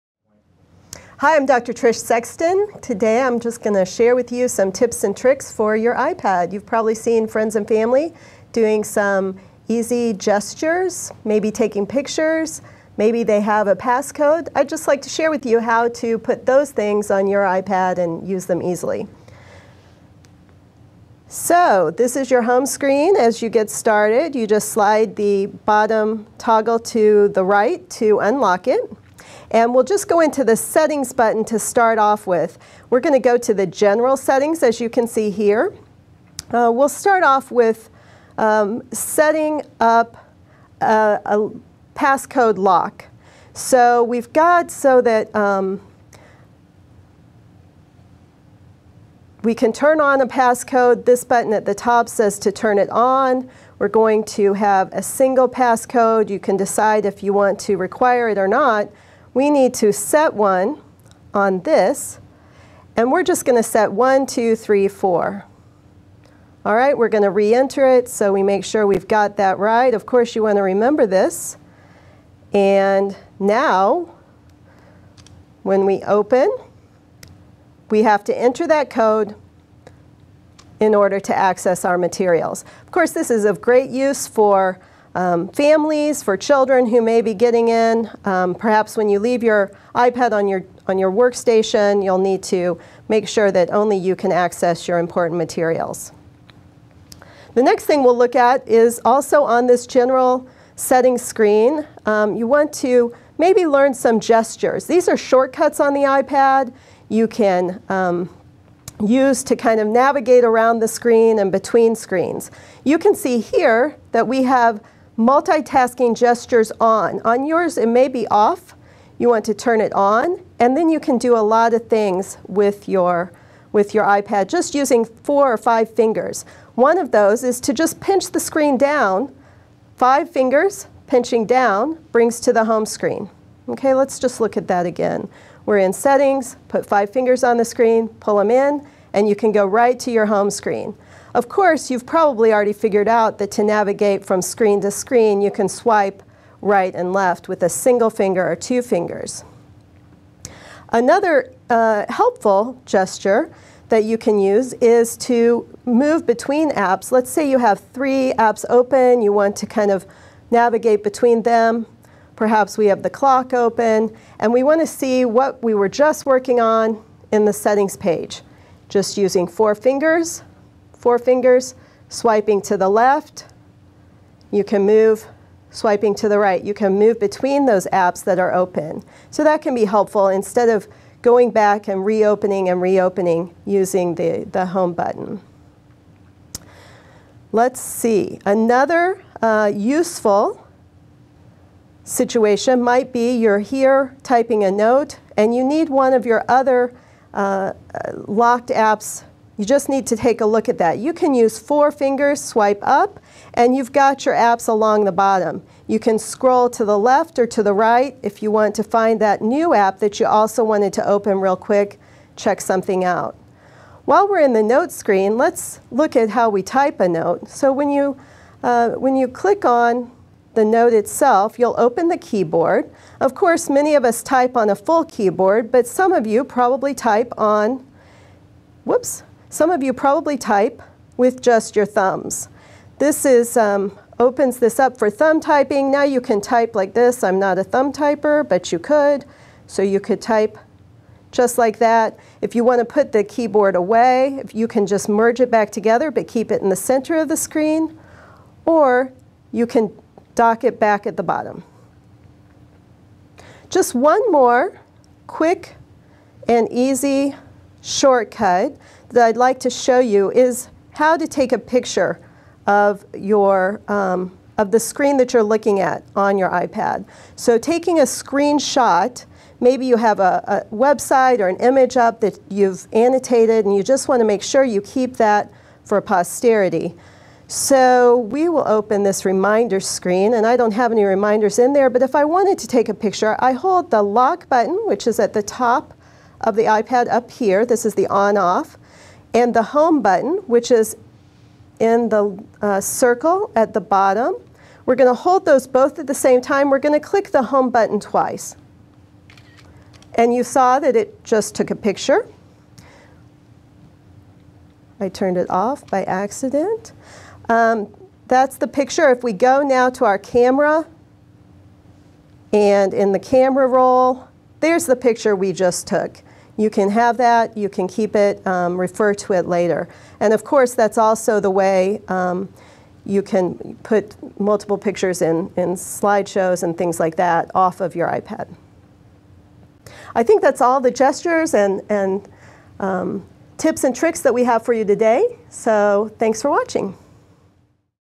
iPad Tutorial: How to Enable Passcode, Multitasking Gestures, and Take a Screen Shot
Trish Sexton, DHEd, Associate Dean of Curriculum at KCOM provides a walk-through on how to enable pass code preferences on your iPad, and also covers multitasking gestures and how to take a screen shot using your iPad.